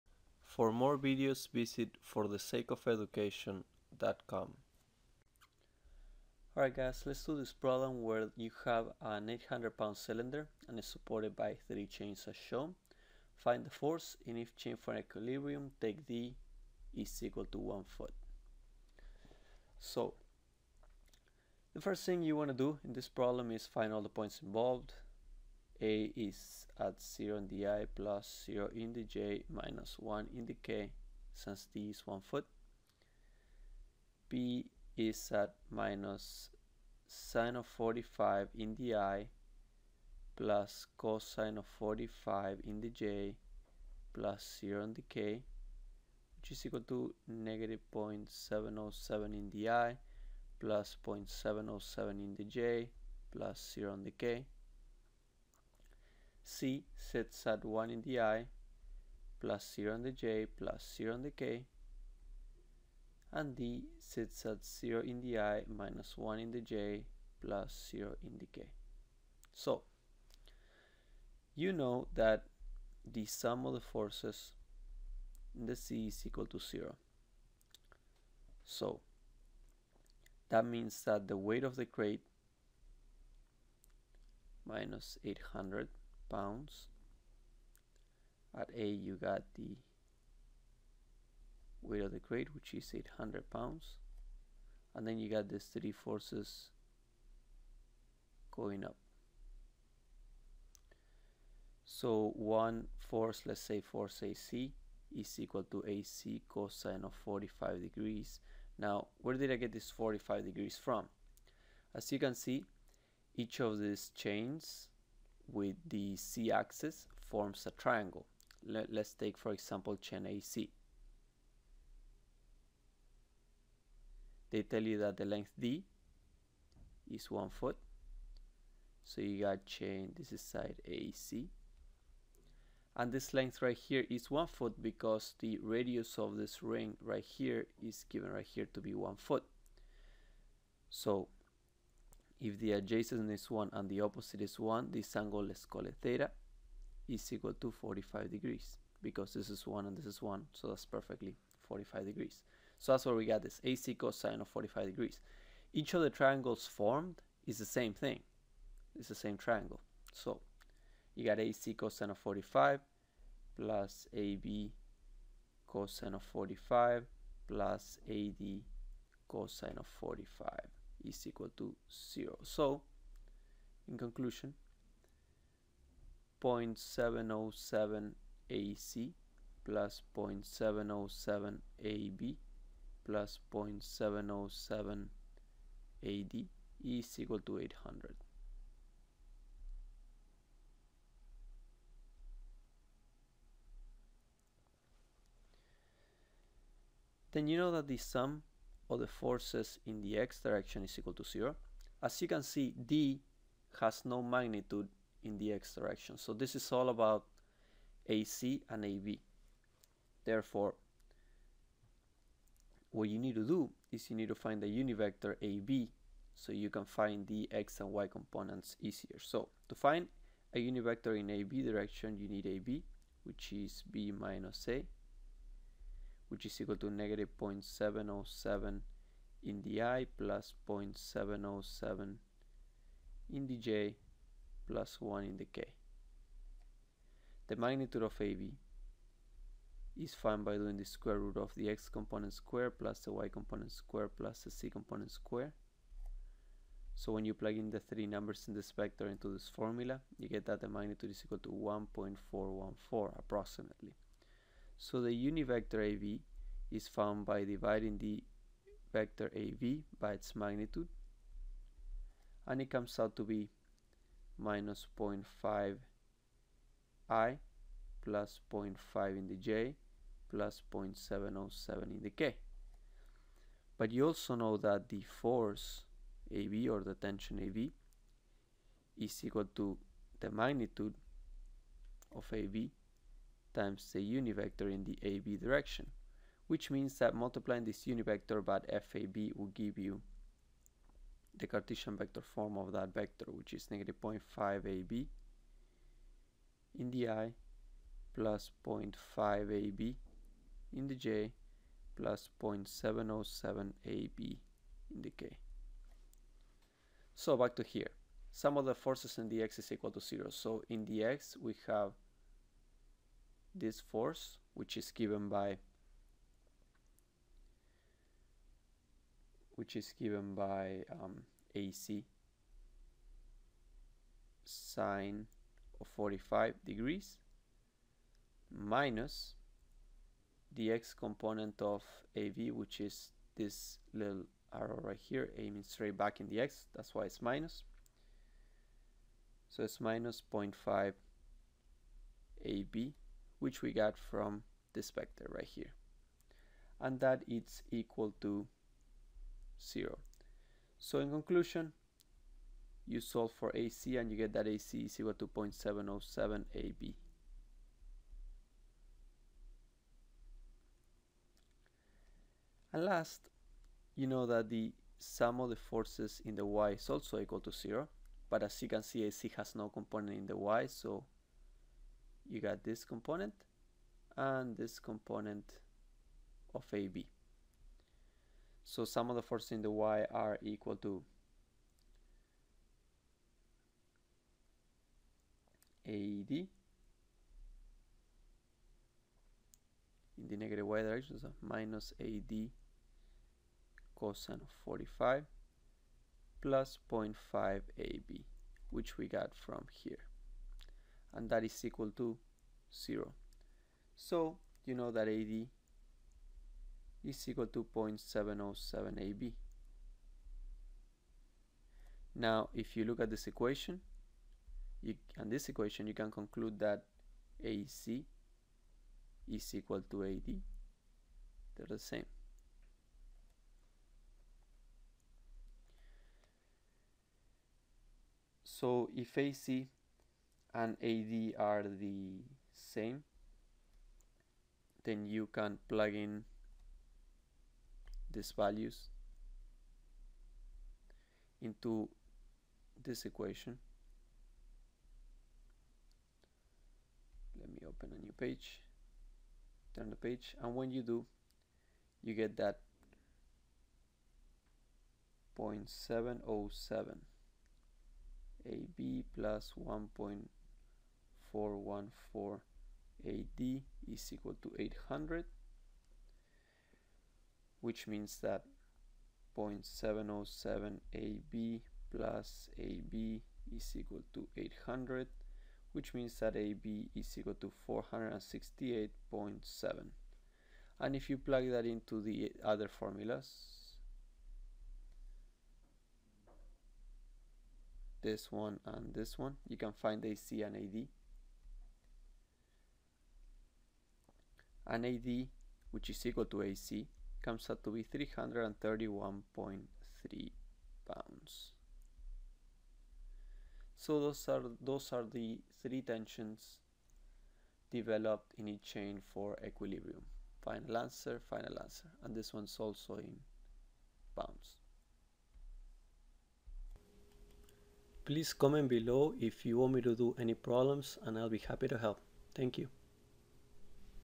Determine the force in each chain for equilibrium. Take d = 1 ft
The 800-lb cylinder is supported by three chains as
shown. Determine the force in each chain for equilibrium.
Take d = 1 ft. Get the book